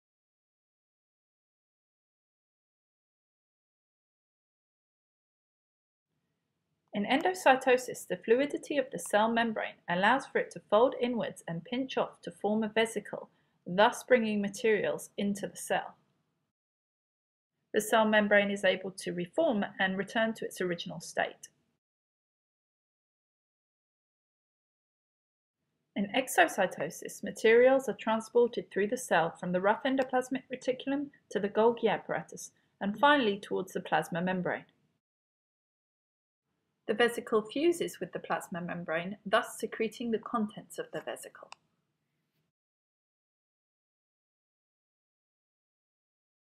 B2.1.13 Endocytosis and Exocytosis (Higher Level)
This video provides addresses standard B2.1.13 (Higher Level) for the new IB Biology syllabus with first exams 2025.